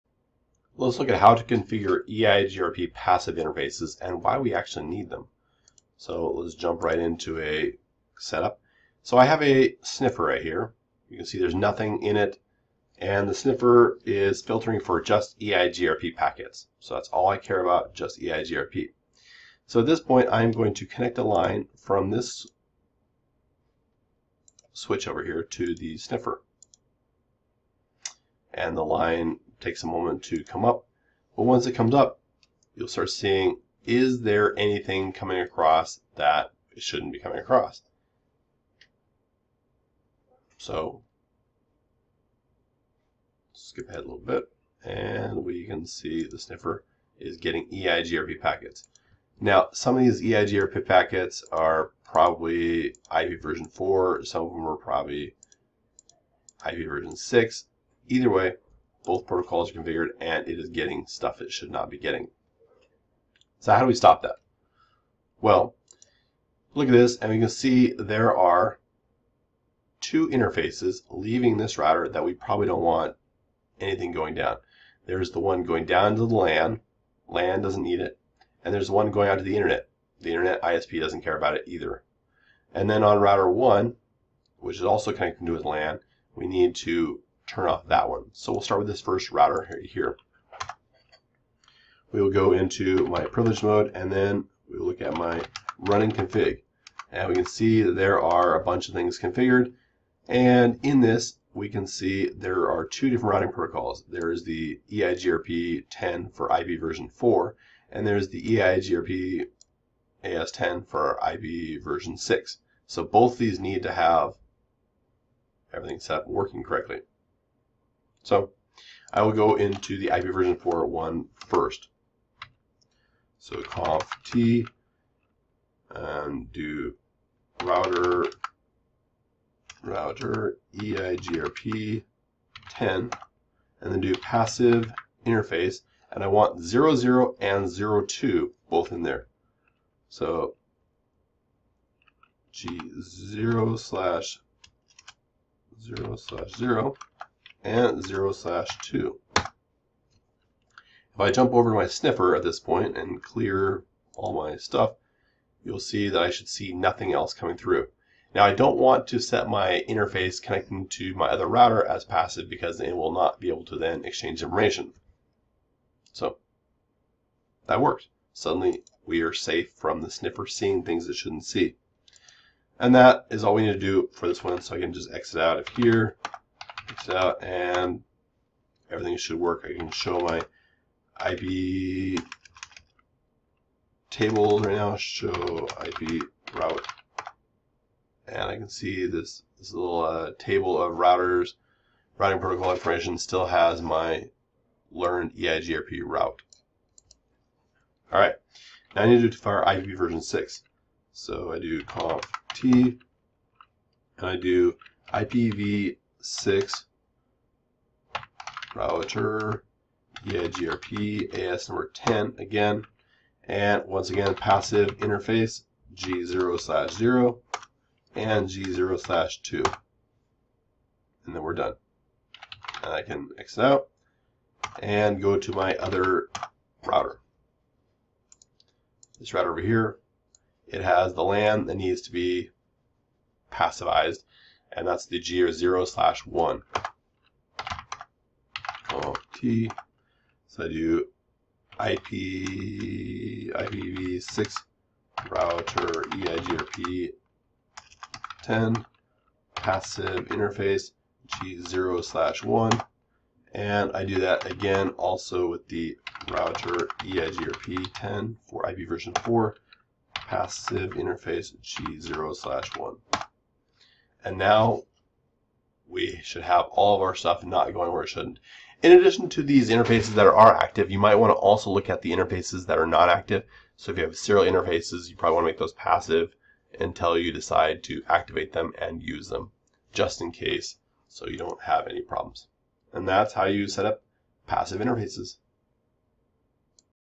PacketTracer - Configuring EIGRP Passive Interfaces
Show how to take an existing EIGRP configuration and set passive interfaces.